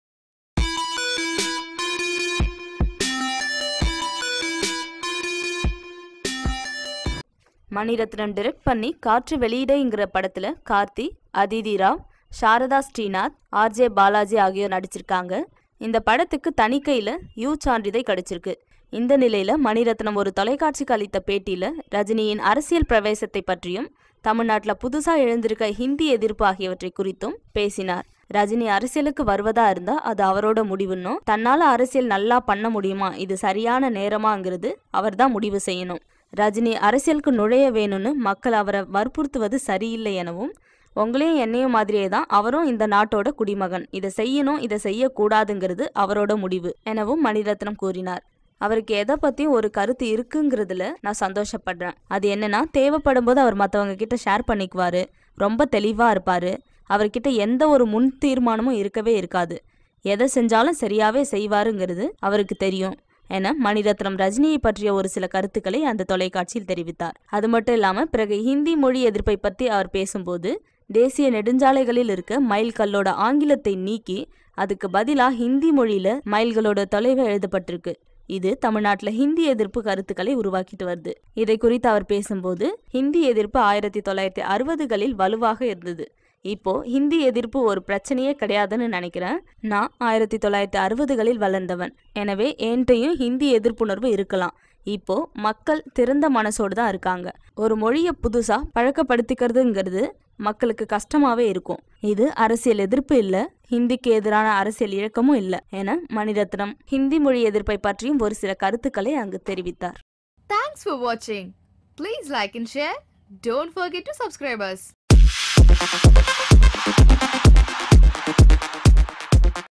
Manirathnam Says about Anti-Hindi Activities and Rajini's Politic Travel!
ரஜினியின் அரசியல் பிரவேசம், புதிய ஹிந்தி எதிர்ப்பு: மணி ரத்னம் என்ன சொல்கிறார்?
மணி ரத்னம் இயக்கியுள்ள காற்று வெளியிடை படத்தில் கார்த்தி, அதிதி ராவ், ஷ்ரதா ஸ்ரீநாத், ஆர்.ஜே. பாலாஜி போன்றோர் நடித்துள்ளார்கள். இப்படத்துக்குத் தணிக்கையில் யு சான்றிதழ் கிடைத்துள்ளது.
இந்நிலையில் ஒரு தொலைக்காட்சிக்கு அளித்த பேட்டியில் ரஜினியின் அரசியல் பிரவேசம் மற்றும் தமிழ்நாட்டில் புதிதாக எழுந்துள்ள ஹிந்தி எதிர்ப்பு ஆகியவை குறித்து இயக்குனர் மணிரத்னம் கூறியதாவது:
ரஜினி அரசியலுக்கு வருவதாக இருந்தால் அது அவருடைய முடிவு. தன்னால் அரசியலில் நன்றாக பண்ணமுடியுமா, இது சரியான நேரமா என்பதை அவர்தான் முடிவு செய்யவேண்டும். ரஜினி அரசியலுக்குள் நுழையவேண்டும் என மக்கள் அவரை வற்புறுத்துவது சரியல்ல. உங்களையும் என்னையும் போல அவரும் இந்த நாட்டின் குடிமகன். இதைச் செய்யவேண்டும், செய்யக்கூடாது என்பது அவர் முடிவு. அவருக்கு எதைப் பற்றியும் ஒரு கருத்து உண்டு என்பதில் சந்தோஷப்படுகிறேன். தேவைப்படும்போது மற்றவர்களிடம் பகிர்ந்துகொள்கிறார். மிகவும் தெளிவாக இருக்கிறார். அவரிடம் எந்தவொரு முன்தீர்மானங்களும் இல்லை. எதைச் செய்தால் சரியாக இருக்கும் என்பது அவருக்குத் தெரியும் என்று கூறியுள்ளார்.
தேசிய நெடுஞ்சாலைகளில் உள்ள மைல் கல்களில் ஆங்கிலத்தை நீக்கி, அதற்கு பதிலாக ஹிந்தி மொழியில் எழுதப்பட்டுள்ளது. இது தமிழ்நாட்டில் ஹிந்தி எதிர்ப்புக் கருத்துகளை உருவாக்கியுள்ளது. இதுகுறித்து மணி ரத்னம் கூறியதாவது: ஹிந்தி எதிர்ப்பு 1960களில் வலுவாக இருந்தது. இப்போது ஹிந்தி எதிர்ப்பு ஒரு பிரச்னையே கிடையாது என நினைக்கிறேன். நான் 1960களில் வளர்ந்தவன். எனவே என்னிடம் இன்னமும் ஹிந்தி எதிர்ப்புணர்வு இருக்கலாம். இப்போது மக்கள் திறந்த மனத்துடன் உள்ளார்கள். ஒரு மொழியைப் பழக்கப்படுத்திக்கொள்ள மக்களுக்குக் கடினமாகவே இருக்கும். இது அரசியல் எதிர்ப்பு அல்ல. ஹிந்திக்கு எதிரான அரசியல் இயக்கமும் அல்ல என்று கூறியுள்ளார்.